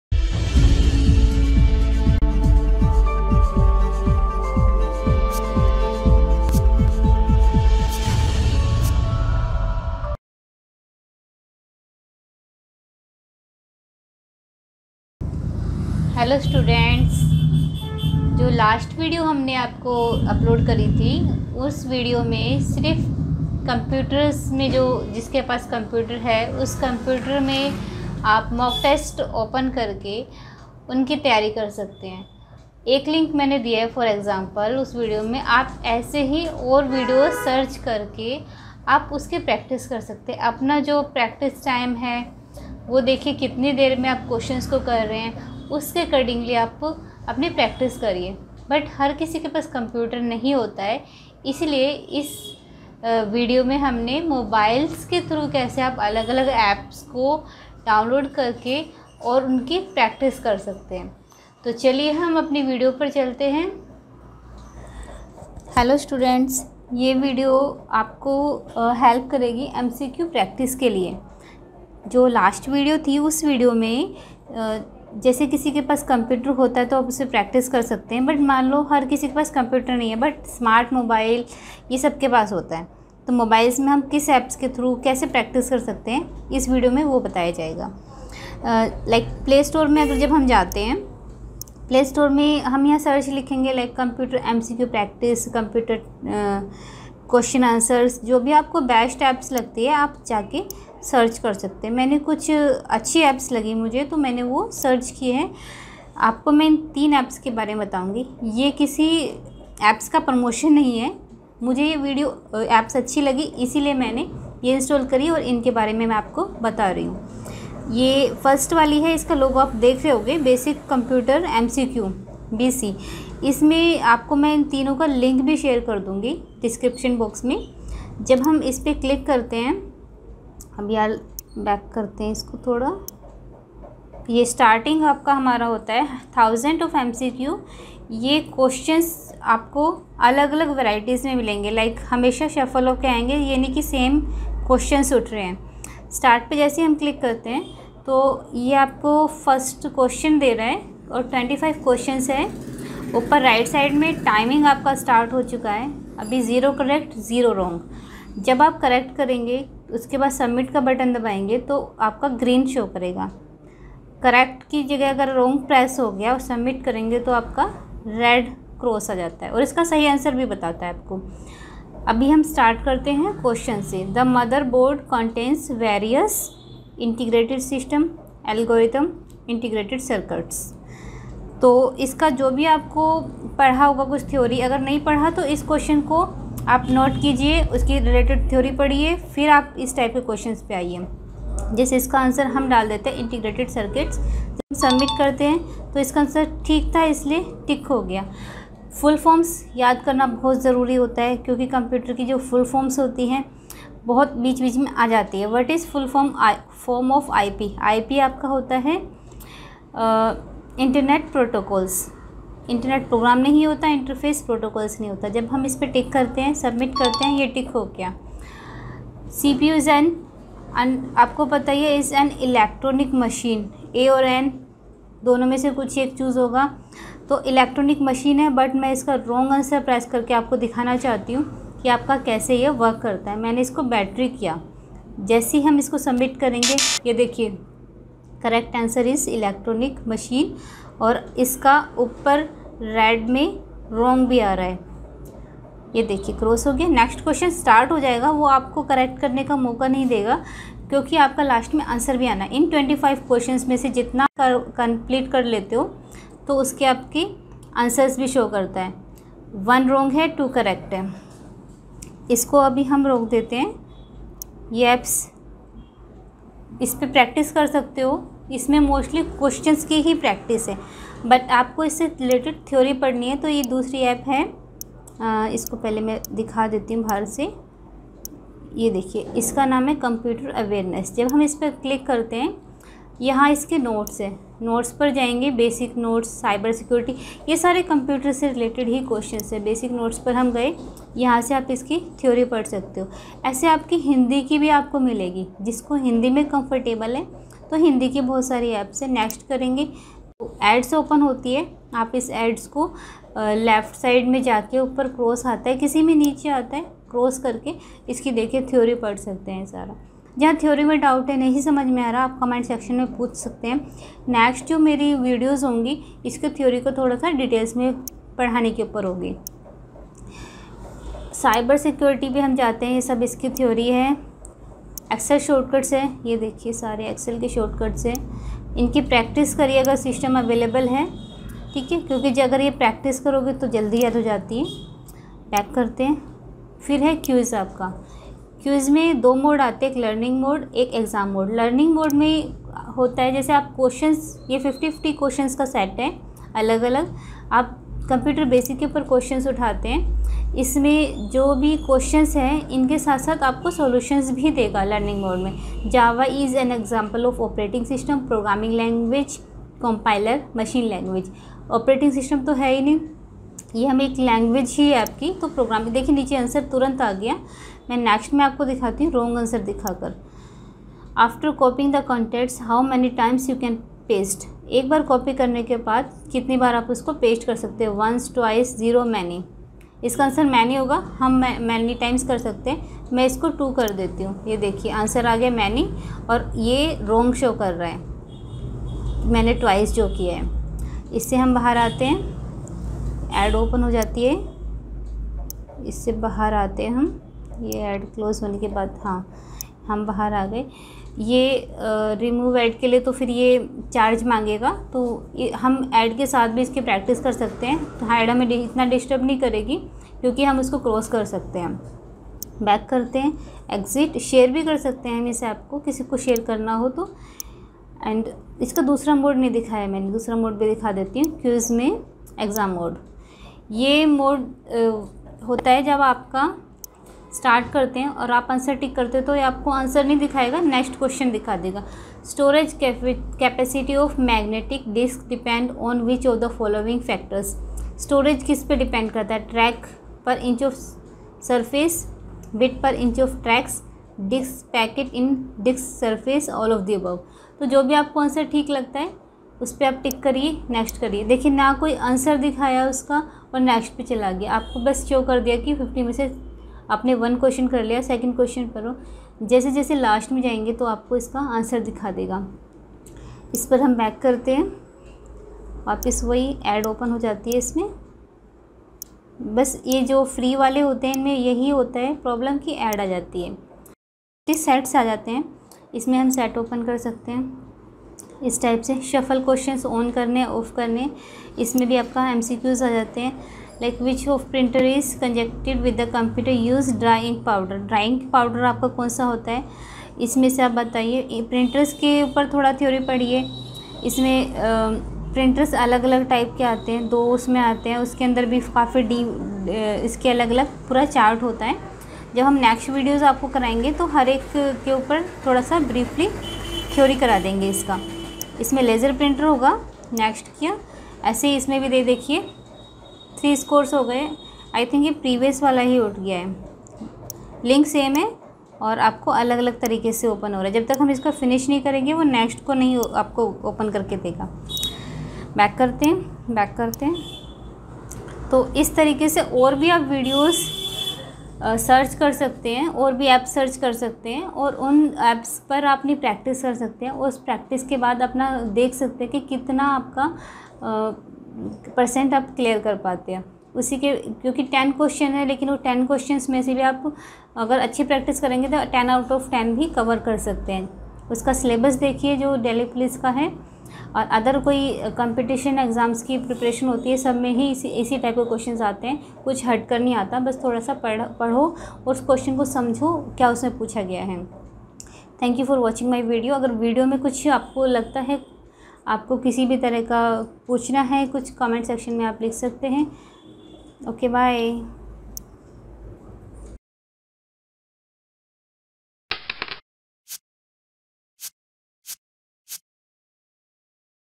Computer MCQ Practice by Mobile APPS
this app is useful for MCQ Computer Practice APPS
some links of given apps in video
1. Basic Computer
2. Computer awareness
3. CCC

This video is about Computers related basic theory and its questions with solutions will comes in all  completions exams lilke ssc, delhipolice etc..

Collar Mic

MICS Computer Education
our institute is established since 2007 we educate students for better job and good computer knowledge